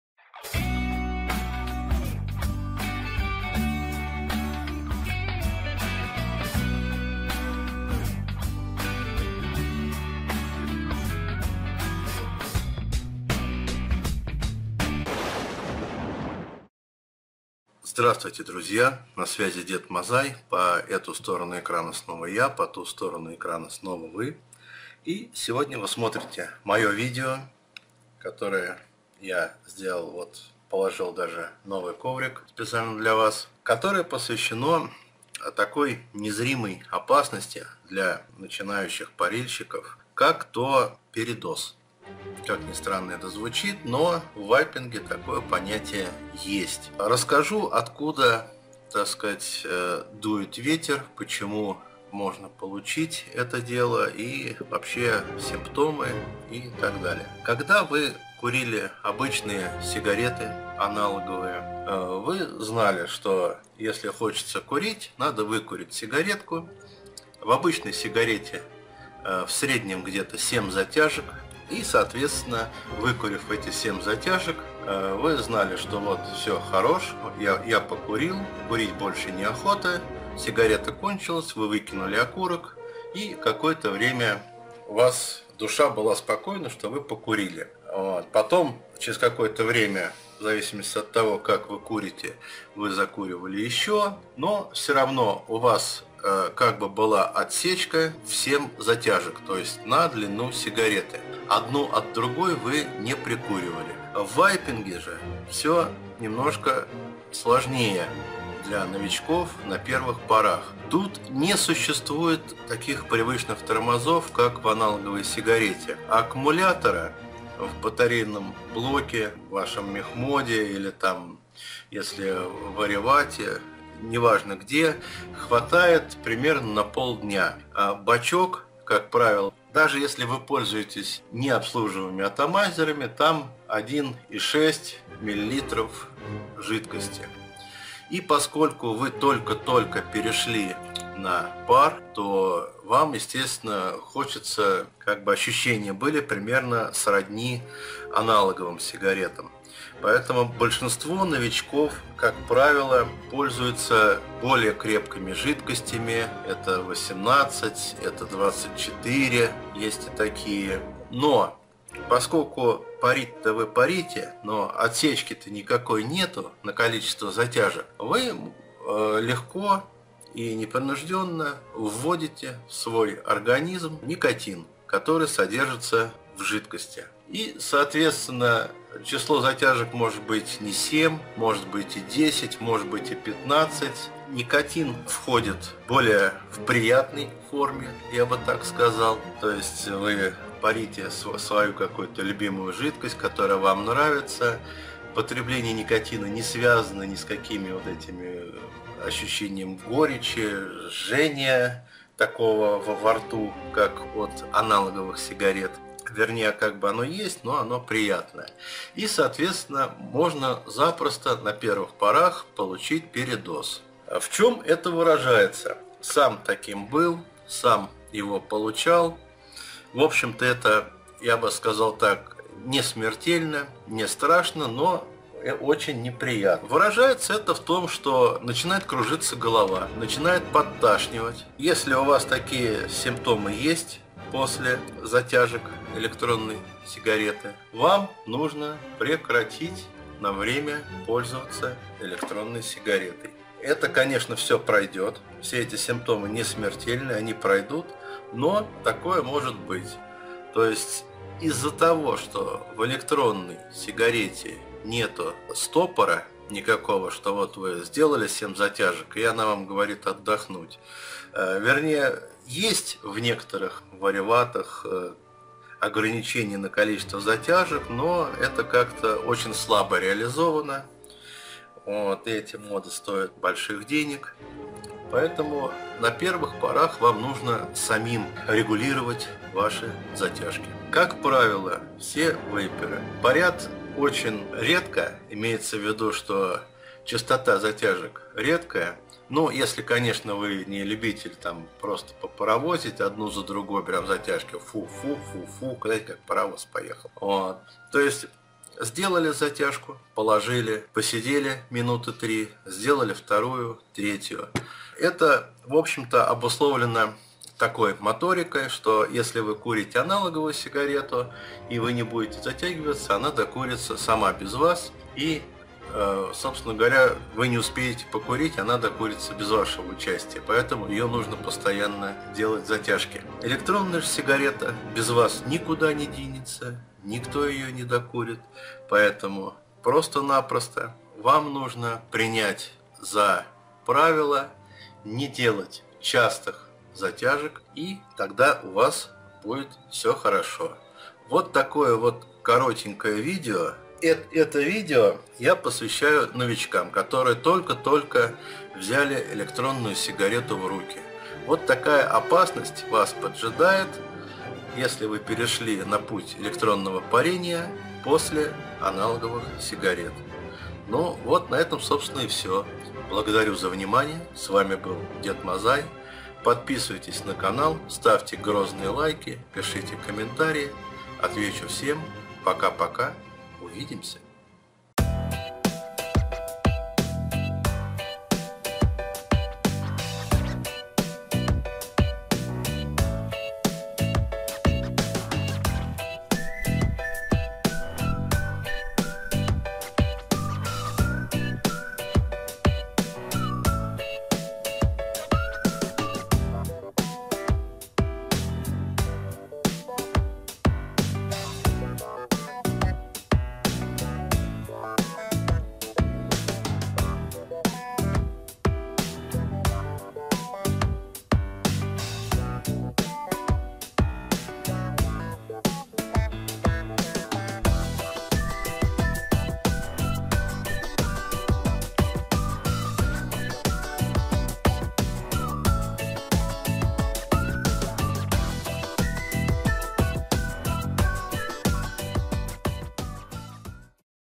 Незримая опасность для начинающего парильщика
Незримая опасность, которая подстерегает практически каждого, кто перешел с обычной сигареты на пар. Что это такое, смотрите в данном видео.